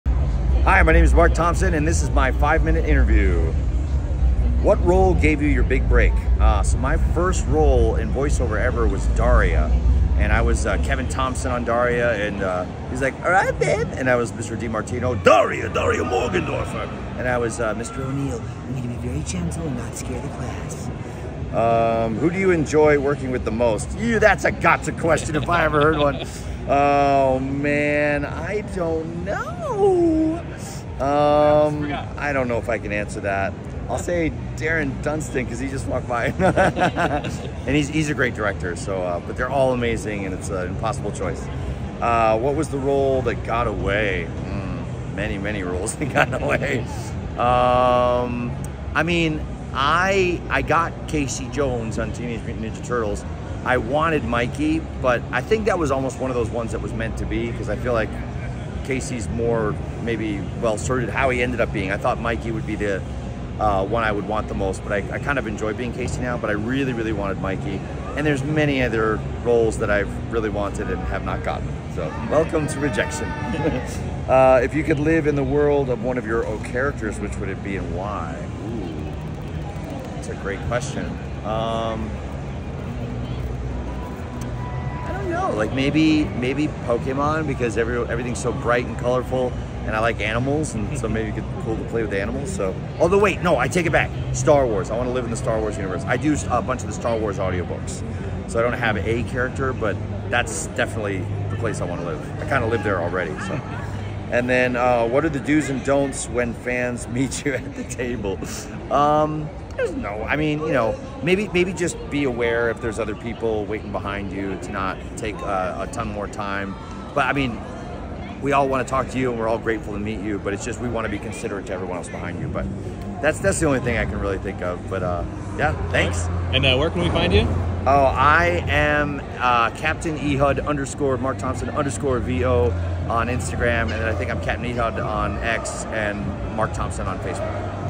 Pokémon Voice Actor Marc Thompson Does the 5-Question Interview at Nostalgia Con 2024
Don’t miss your chance to join Marc Thompson, the prolific voice behind many beloved Pokémon characters, in a special 5-Question Interview at Nostalgia Con 2024. Known for his extensive work across major franchises, Thompson brings a vast array of characters to life, making him a standout talent in the voice-acting community.

In the Pokémon series, Thompson has voiced a variety of characters including Gengar, Ash's Primeape, and multiple others across the expansive Pokémon universe. His contributions extend beyond Pokémon into other significant roles such as Casey Jones in Teenage Mutant Ninja Turtles, various characters in Yu-Gi-Oh!, and voices in Star Wars audiobooks, showcasing his versatility and impact on popular culture.

This intimate interview session will explore Marc Thompson's dynamic career, his approach to voice acting, and behind-the-scenes insights into the making of some of the most iconic series in animation history. Join us at Nostalgia Con 2024 for an unforgettable encounter with one of the giants of voice acting.

👇 BEST SUPPLIES FOR THROUGH THE MAIL (TTM) AUTOGRAPH COLLECTING
1️⃣ 8x10 photo printing
2️⃣.  Forever Stamps
3️⃣.  9.5×12.5 inch envelopes
4️⃣.  9×12 inch return envelopes